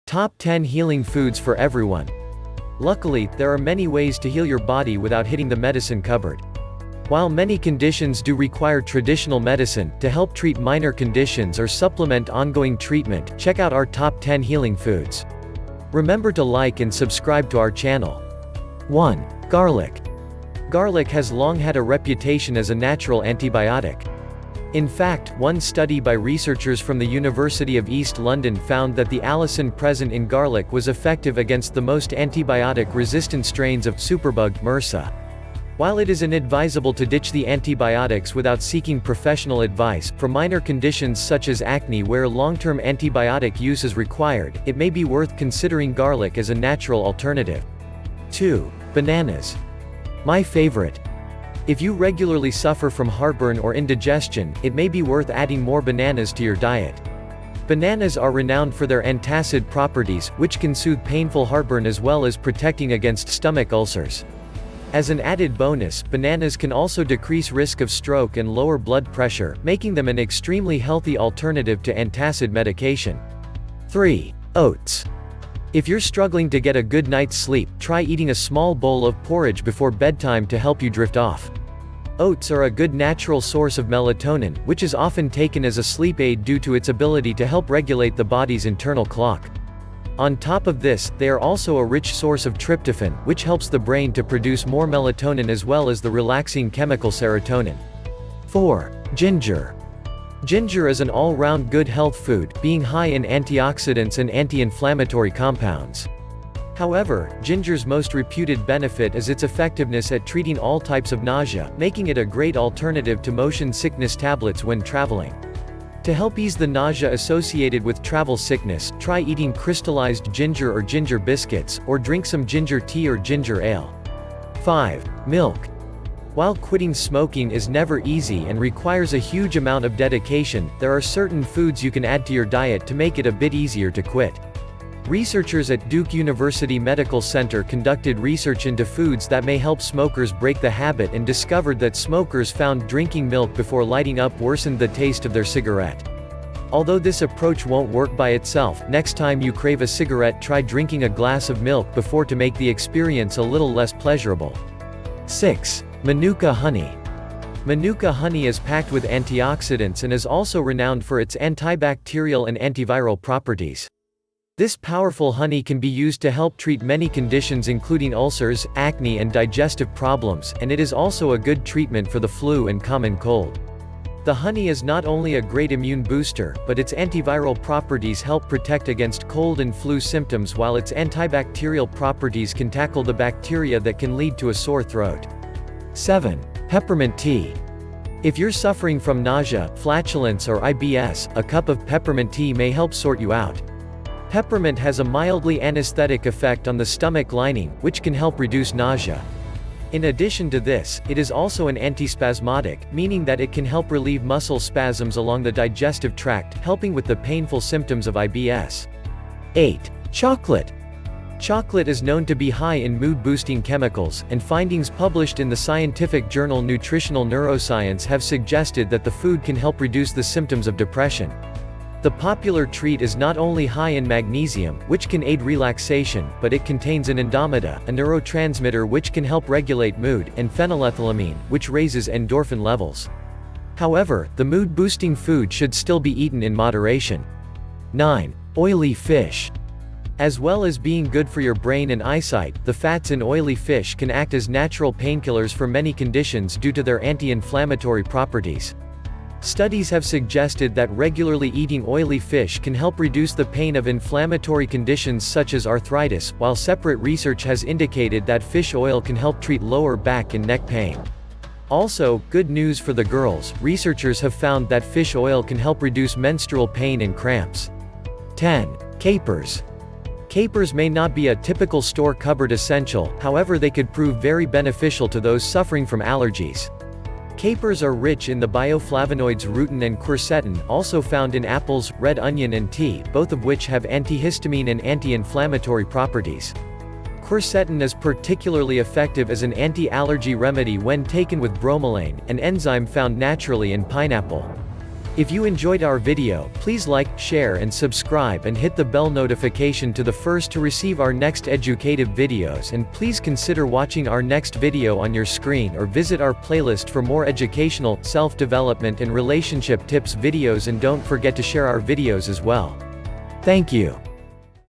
Top 10 Healing Foods For Everyone - Let Thy Food Be Thy Medicine
Healthy food is the medicine we give our bodies every day. Here are Top 10 Healing Foods For Everyone l Let Food Be Thy Medicine.

You Will Never Visit a Doctor Again if You do these three (3) things. - Dr. Anuj